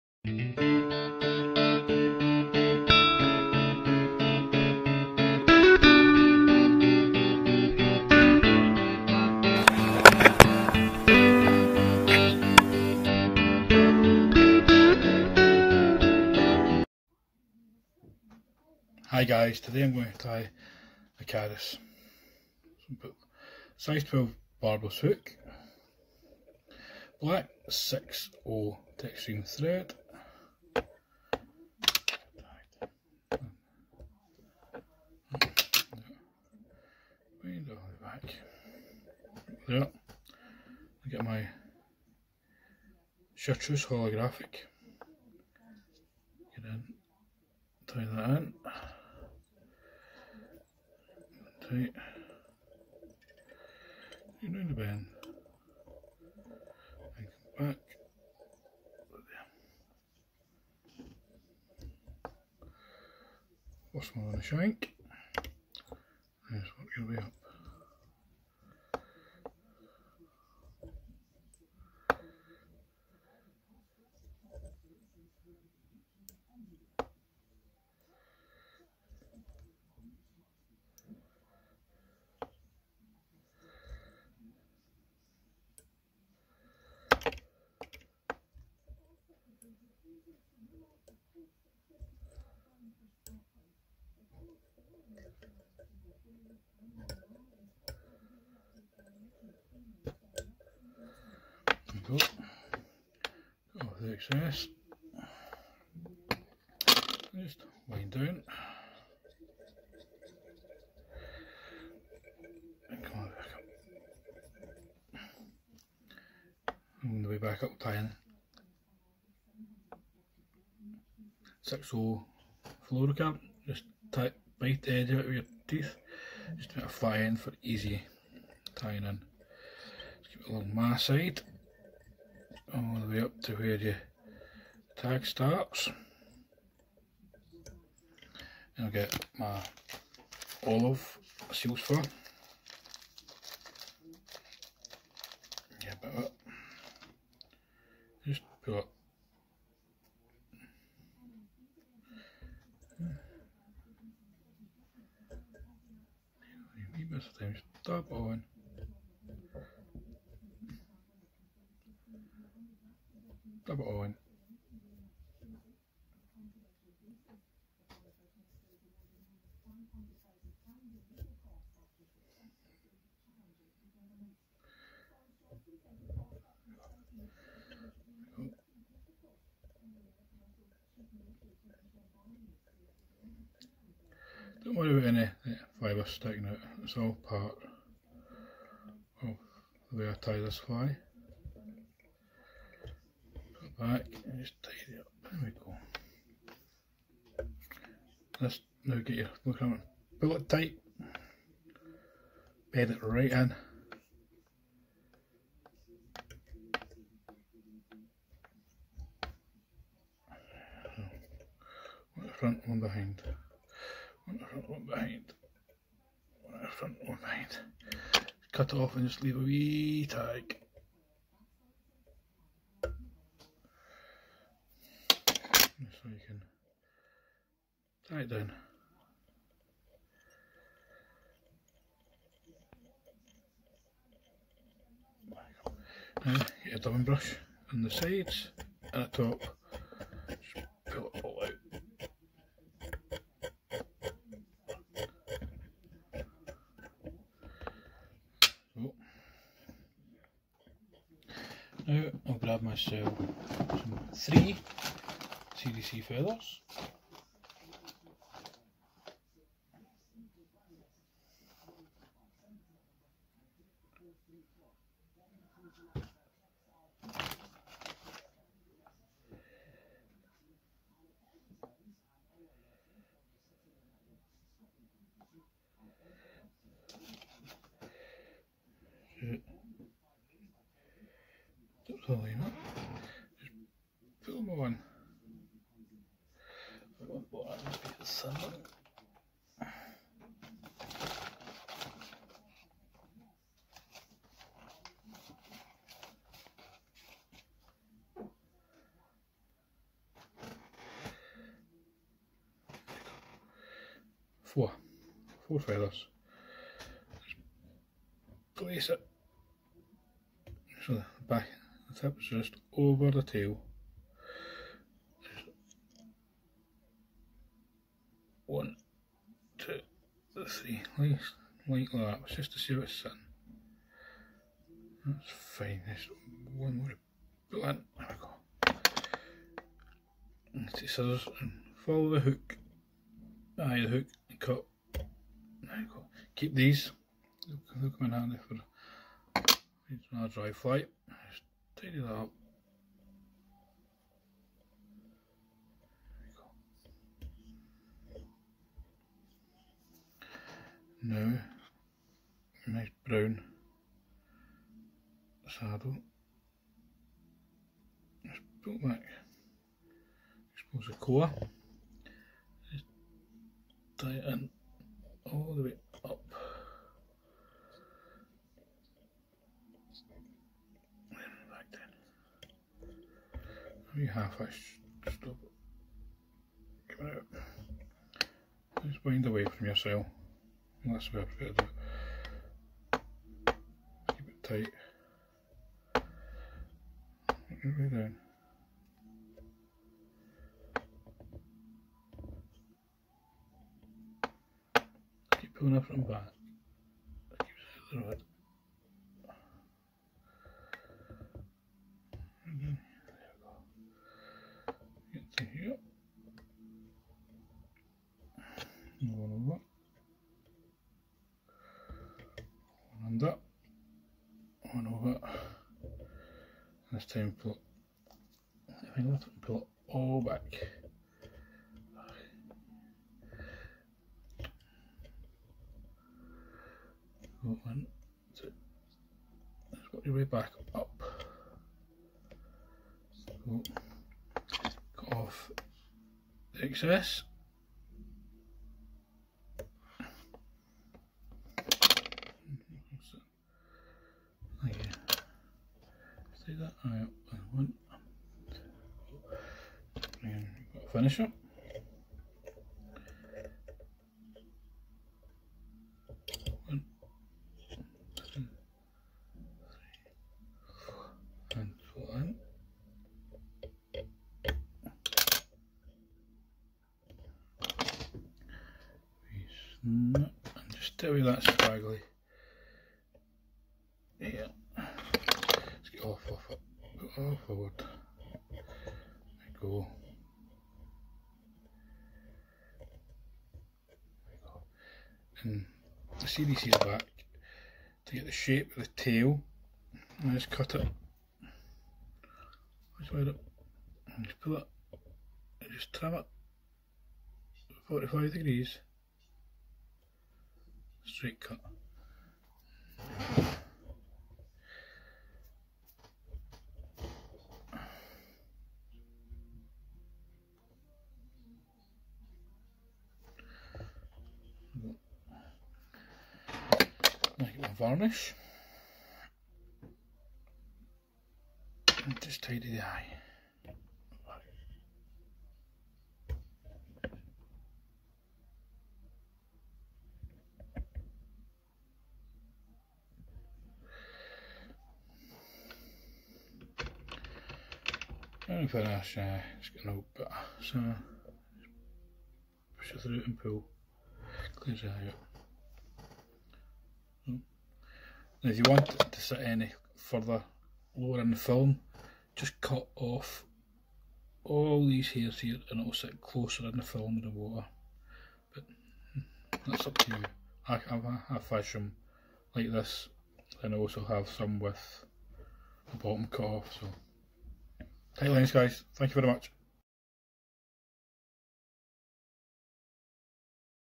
caddis dry fly
Hook - Size 12 wet barbless
Thread - Black 6/0 Textreme
Tag - Chartreuse holographic
Body - Olive seals fur
Rib - 6lb fluorocarbon
Wing - 4 x CDC feathers
Hackle - Brown saddle.